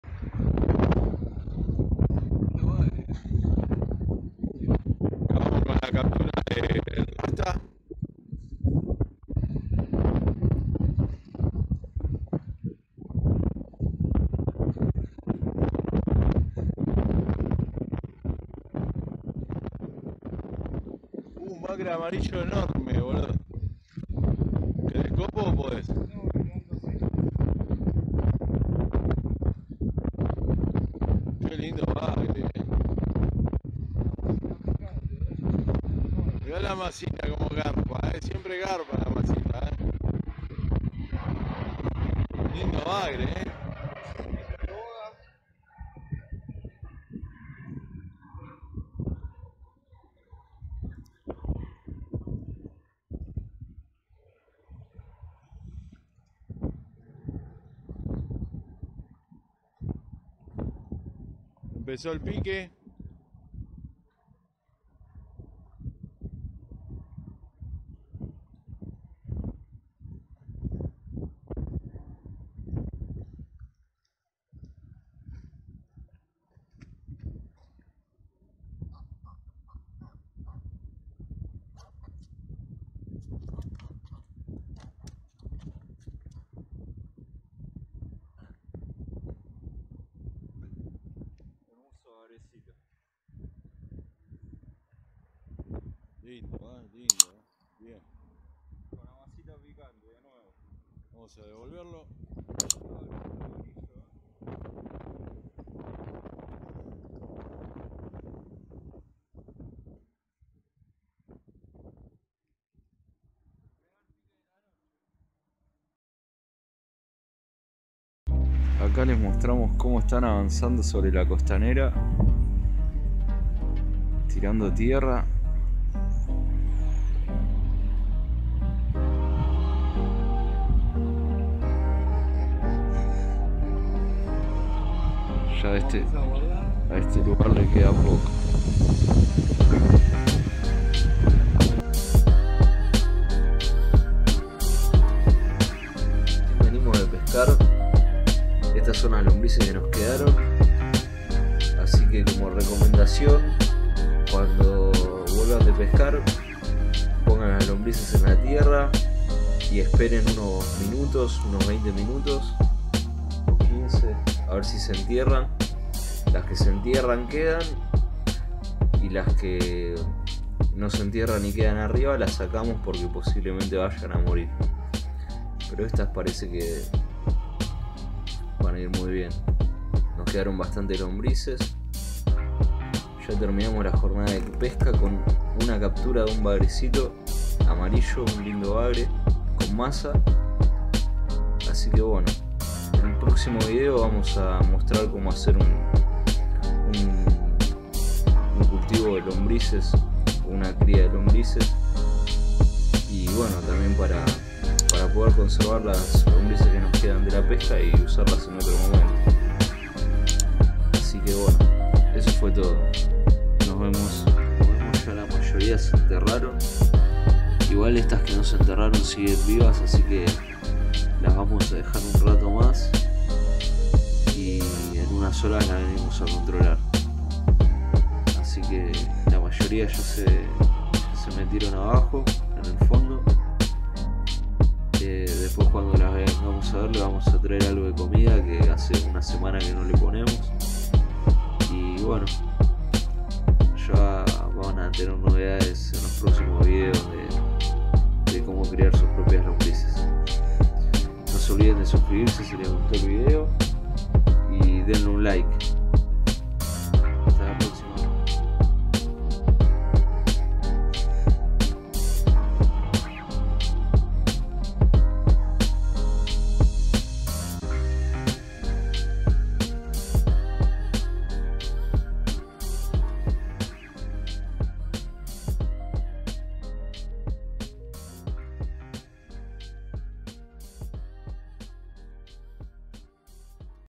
PESCA de BAGRE AMARILLO con MASA!! pescamos en COSTANERA NORTE 2019
Pesca de bagre amarillo en costanera norte,  único pique que se dio con masa picante la misma que usamos para sacar las carpas de los videos anteriores aparentemente le gusta mucho a los bagres amarillos también
Muchas gracias por los comentarios y la buena onda
Buenas pescas para todos